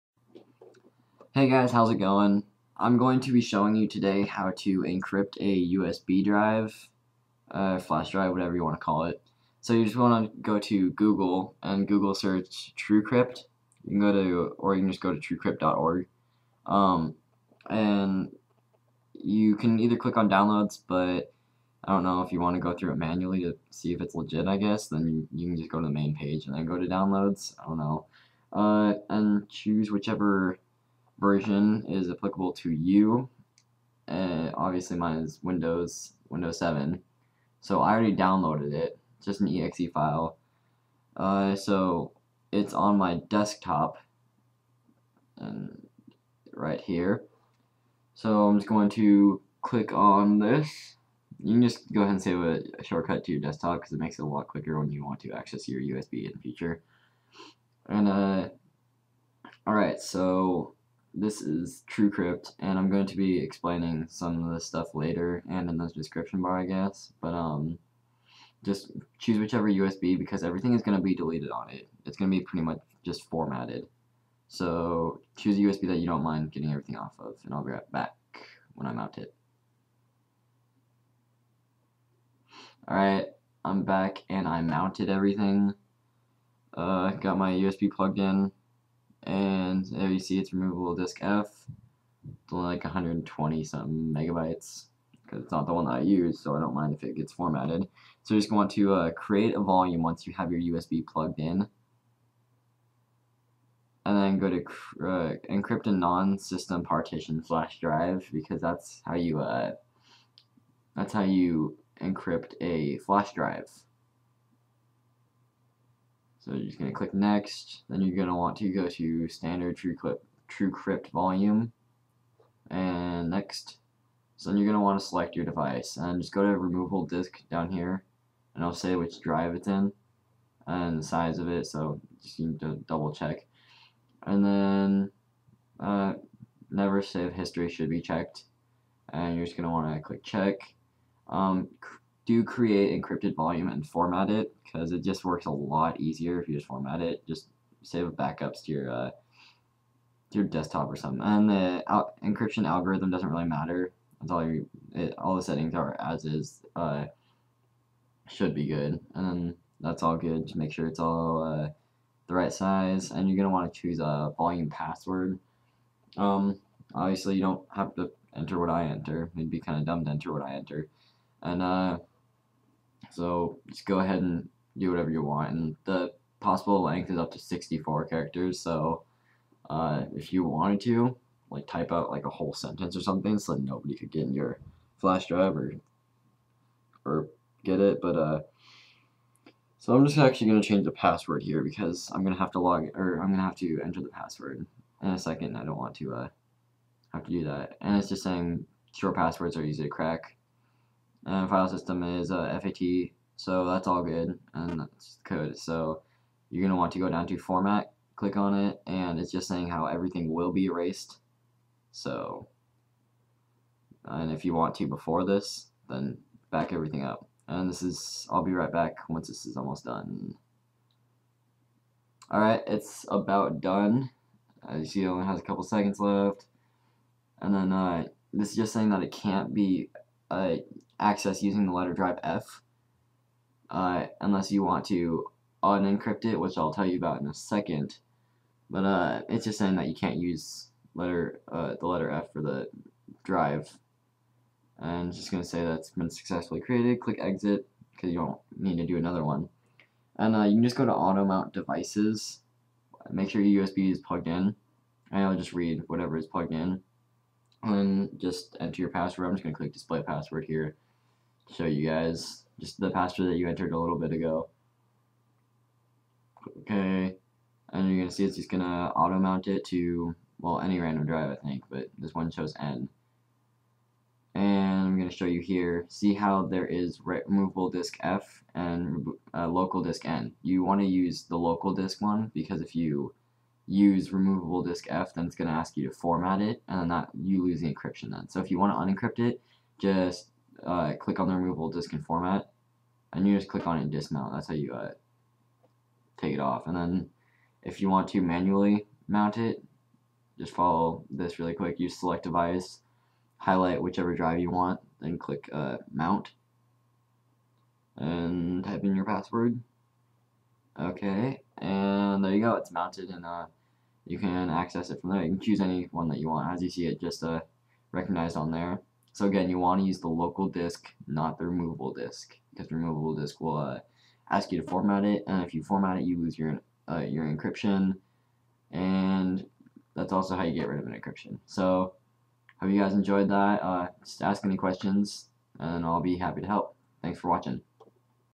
How To Encrypt A USB using TrueCrypt (HD!)
WARNING: THIS FORMATS YOUR FLASH DRIVE!
Here's a video using Truecrypt. It's a completely free program that lets you encrypt flash drives. Easy way to keep files safe!